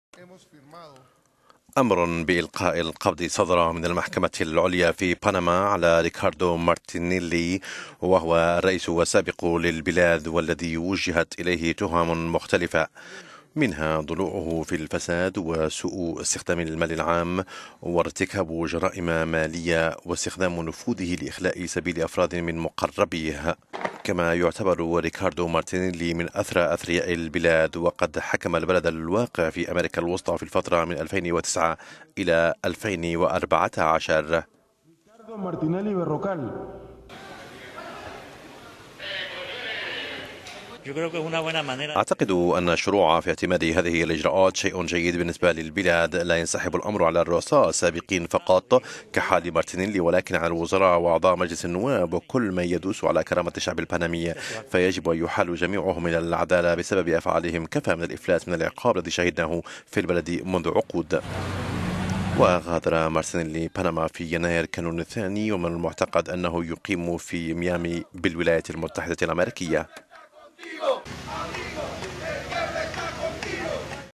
أمر بإلقاء القبض على الرئيس السابق لبنما
أمر بإلقاء القبض صدر من المحكمة العليا في بنما  على  ريكاردو مارتينيلي الرئيس السابق للبلاد والذي وجهت إليه  تهم مختلفة منها ضلوعه في الفساد وسوء استخدام المال العام،وارتكاب جرائم مالية،واستخدام نفوذه لإخلاء سبيل أفراد من مقربيه فضلا عن تقاضيه رشى،والتجسس بطريقة غير قانونية على أكثر من 150 شخصا وتتبع نشطاء وسياسيين وأعضاء نقابيين ومحامين وأطباء وجماعات مدنية أخرى.
ويعتبر  ريكاردو مارتينيلي من أثرى أثرياء البلاد وقد حكم البلد الواقع في أمريكا الوسطى في الفترة من 2009 إلى 2014 .